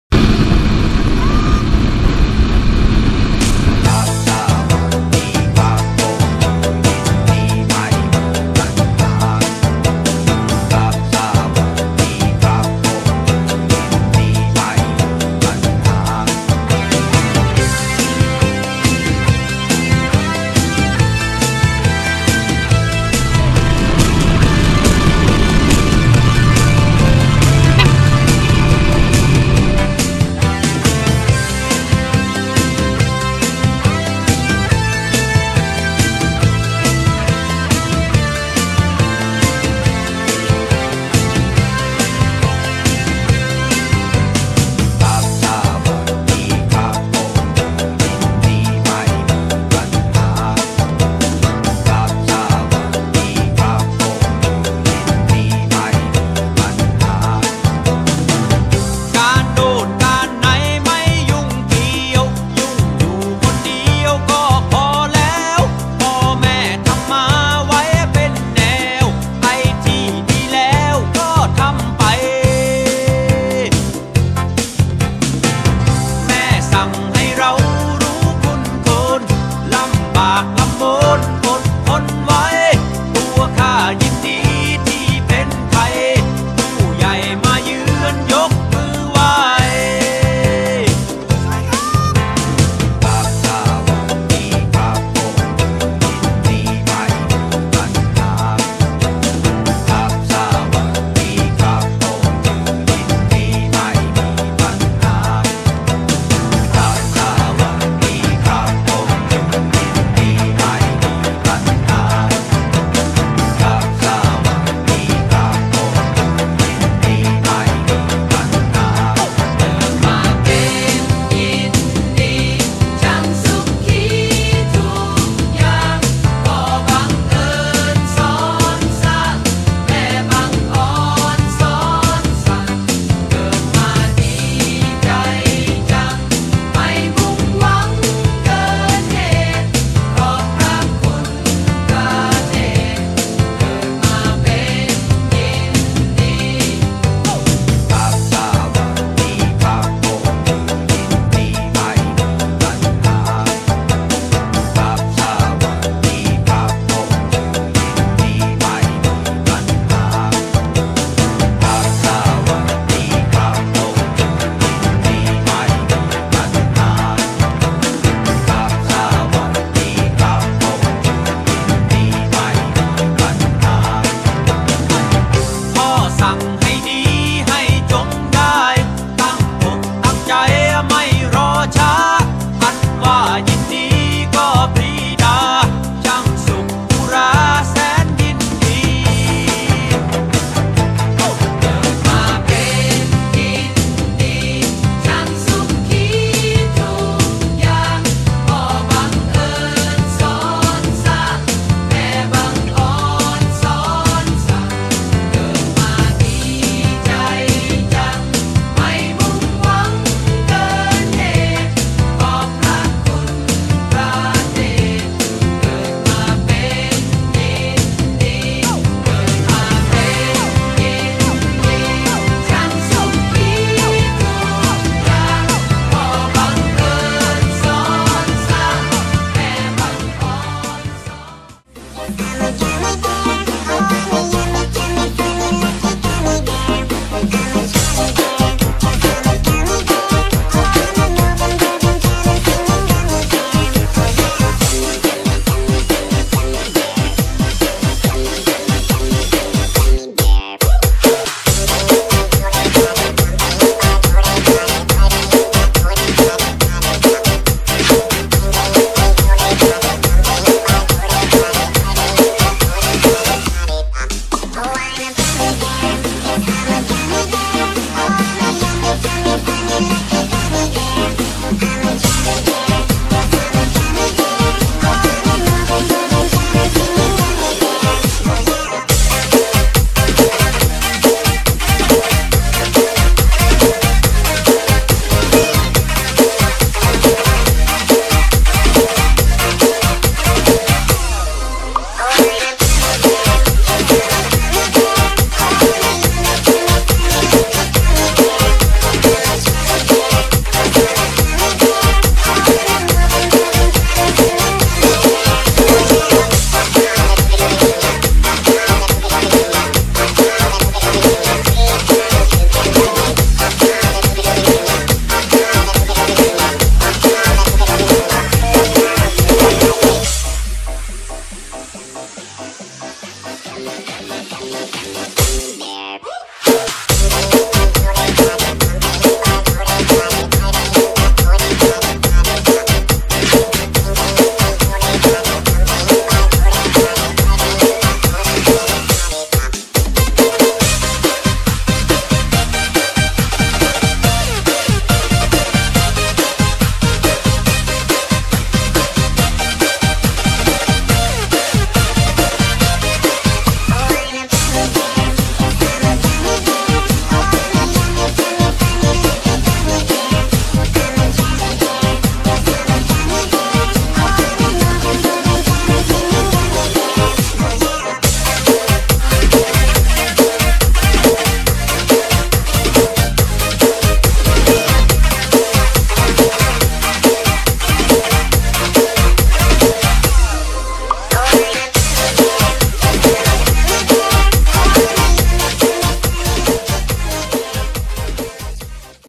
Y15 dan Geng Ride ke Burapa Bike Week, Pattaya, Thailand 2019
Ini merupakan Hari Ke-5 dlm siri kembara motosikal Y15 dan kawan2 dari Kedah ke Bangkok dan terus ke Pattaya utk memeriahkan Burapa Bike Week.